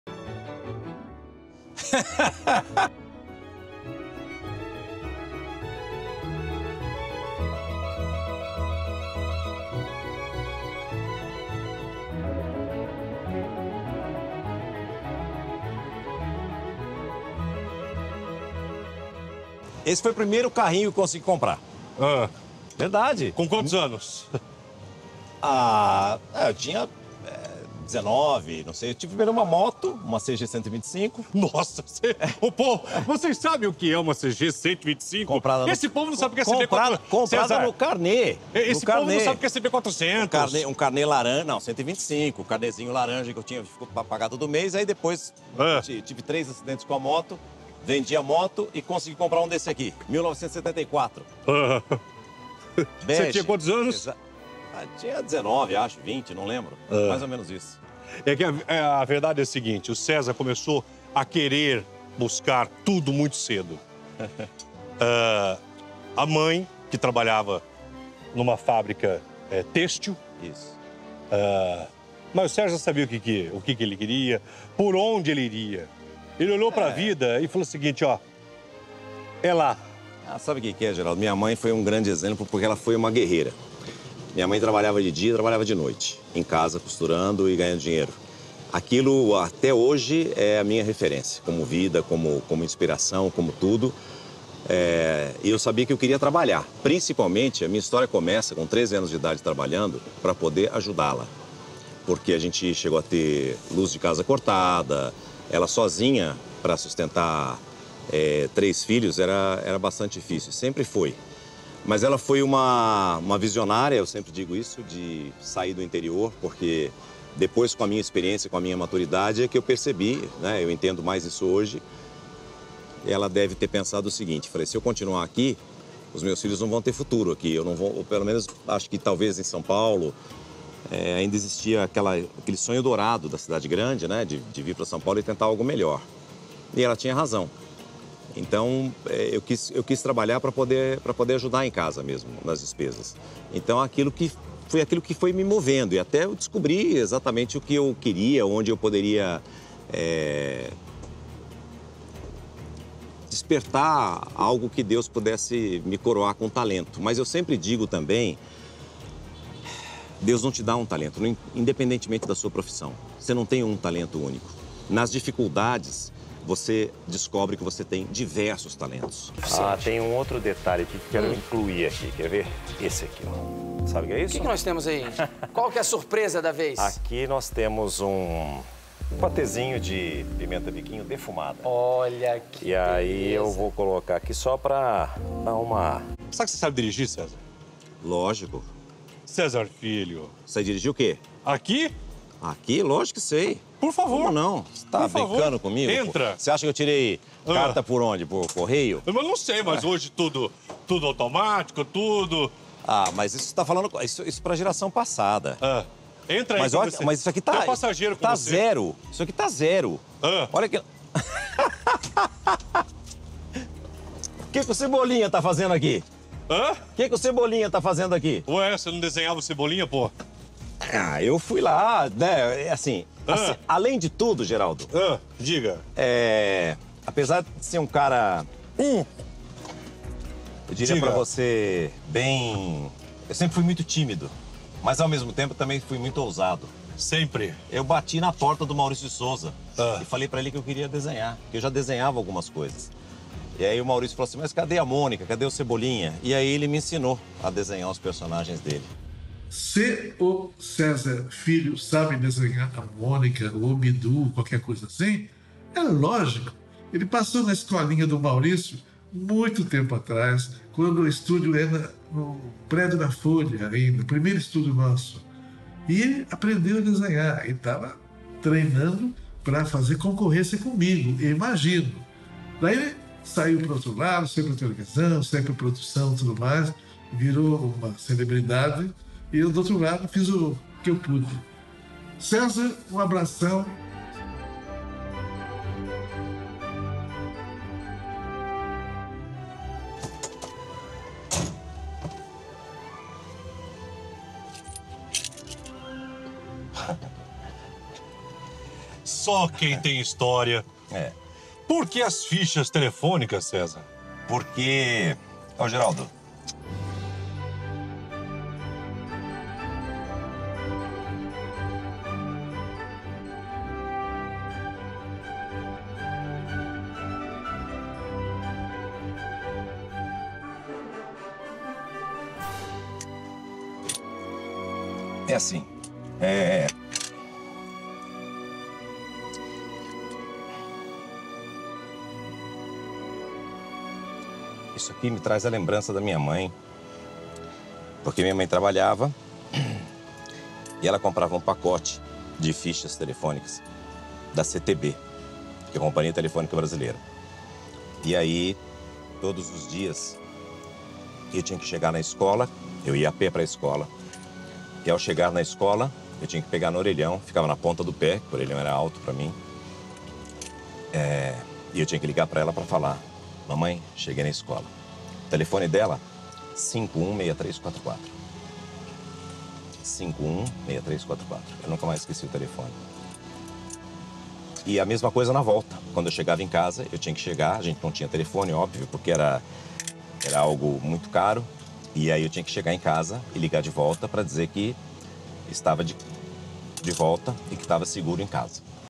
A trajetória e momentos difíceis que Cesar Filho viveu
Dentro de um carro que é do mesmo modelo do primeiro que ele comprou na vida, Cesar Filho conta para Geraldo Luís como foi a infância e relembra que decidiu trabalhar para ajudar a mãe nas despesas. Para assistir ao conteúdo na íntegra, acesse